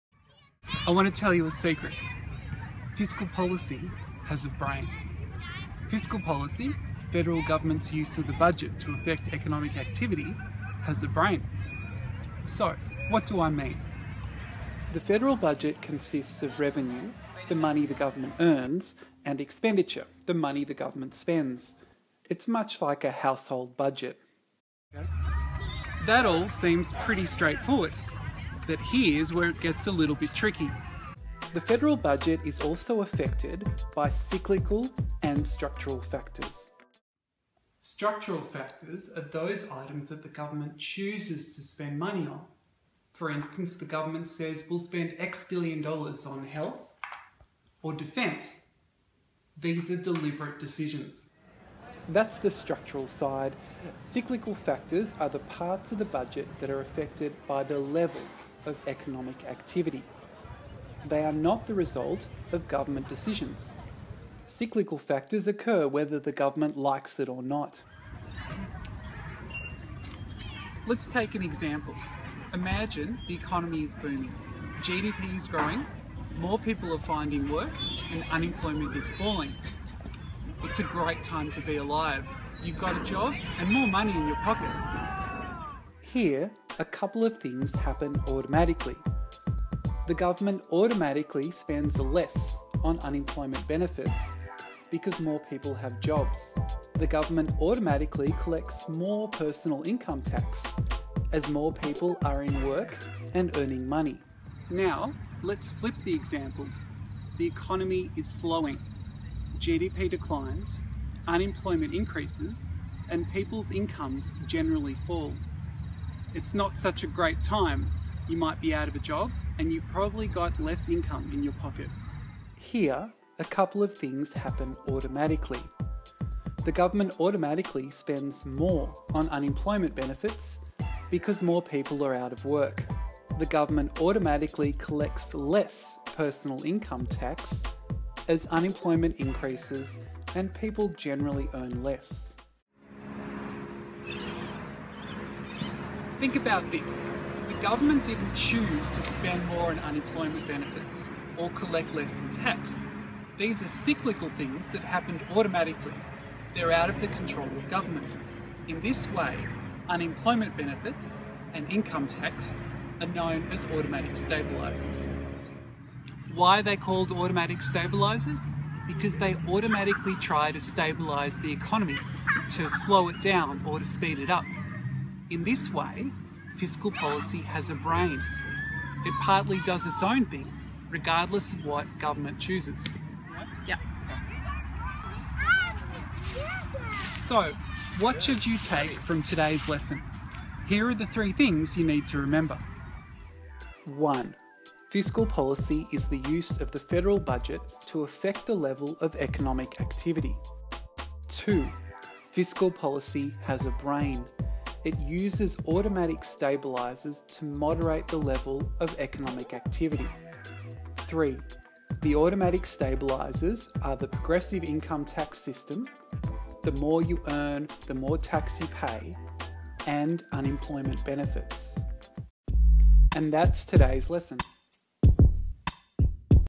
All about automatic stabilisers
Confused by the economics concept of automatic stabilisers? Try this video. It takes you through the basics of fiscal policy, and how automatic stabilisers work.